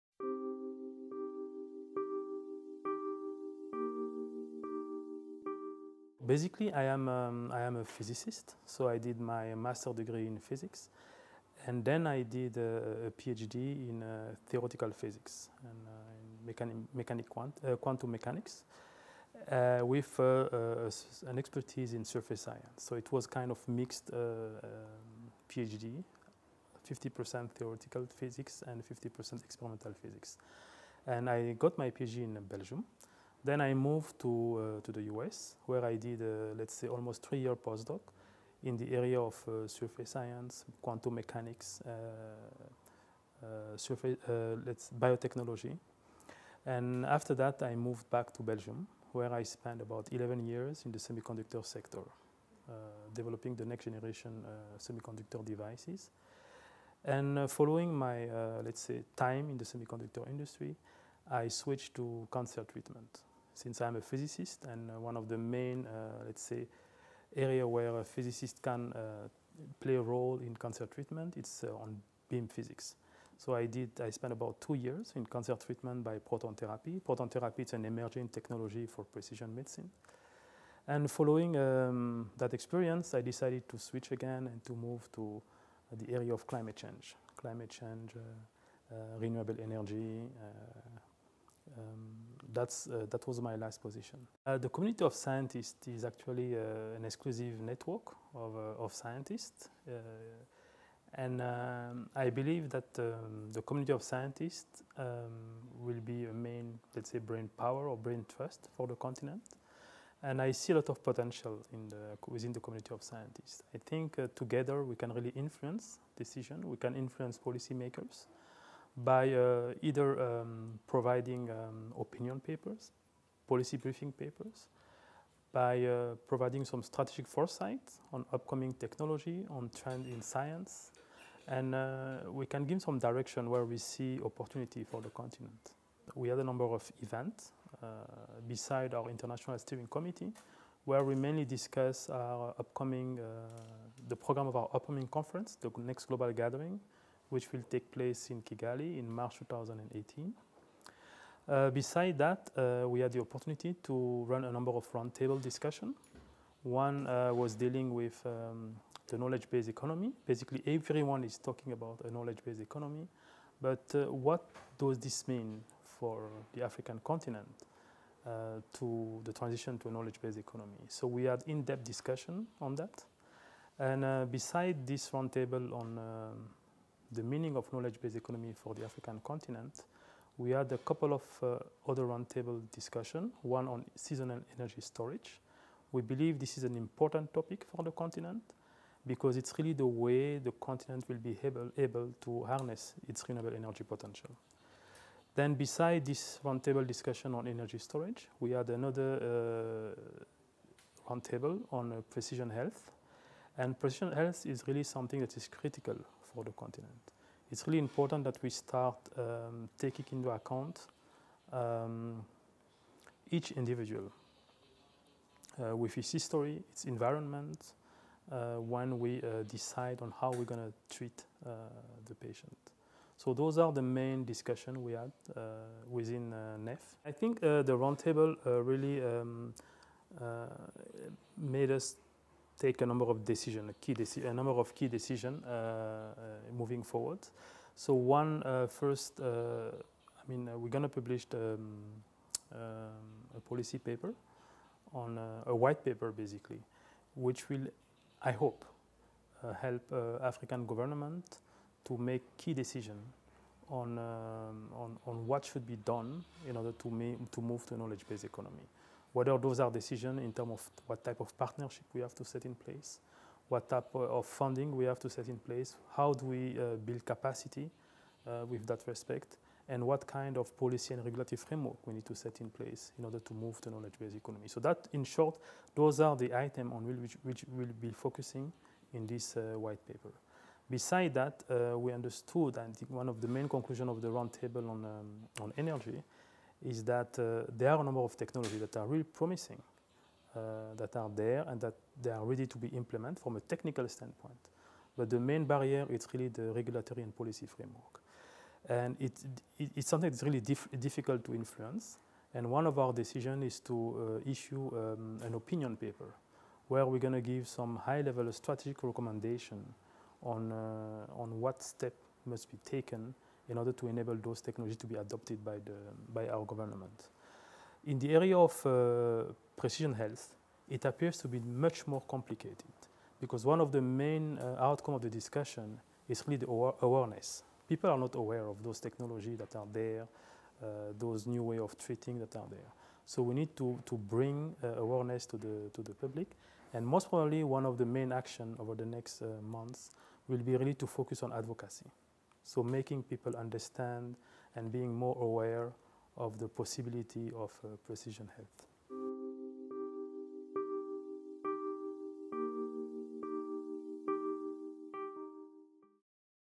Dr. Youssef Travaly, NEF's Lead Scientist
Get to know the NEF's Director of Programs and Content and Lead Scientist, Dr. Youssef Travaly. Dr. Travaly discusses his own scientific journey and introduces the NEF Community of Scientists.